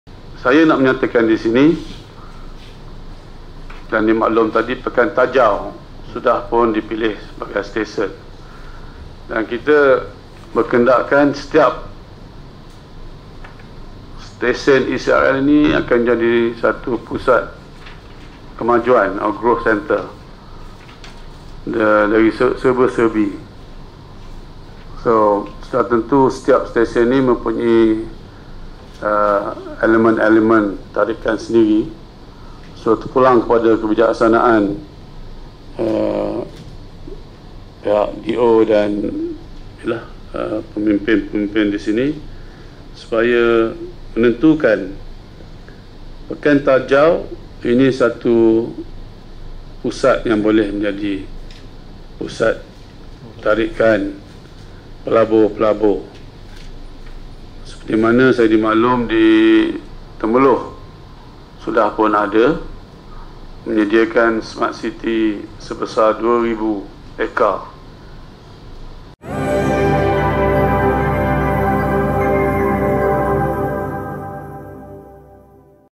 SULTAN PAHANG TITAH WUJUDKAN PUSAT KEMAJUAN DI KAWASAN STESEN ECRL
|Sedutan Video| SULTAN PAHANG TITAH WUJUDKAN PUSAT KEMAJUAN DI KAWASAN STESEN ECRL